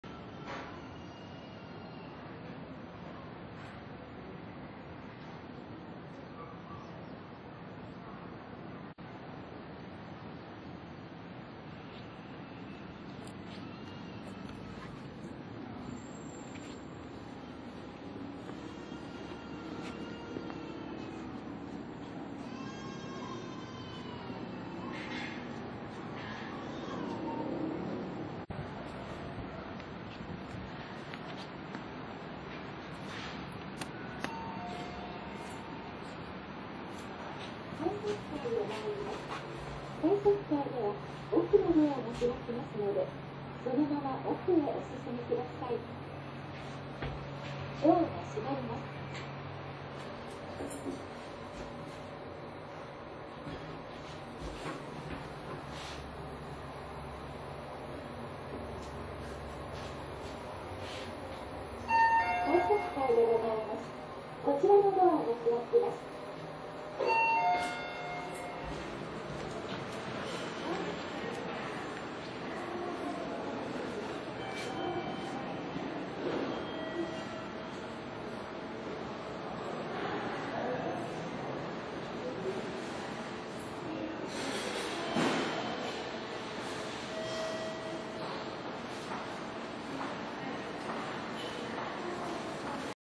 駅エレベーター 日立製 近鉄 東花園駅 新ホーム 奈良線 japan train station Platform Crystal Elevator
近畿日本鉄道の東花園駅(奈良線)の奈良方面に向う新ホームにあるエレベーターです。日立製。

Kintetsu Railway Platform Crystal Elevator at Higashi-Hanazono Station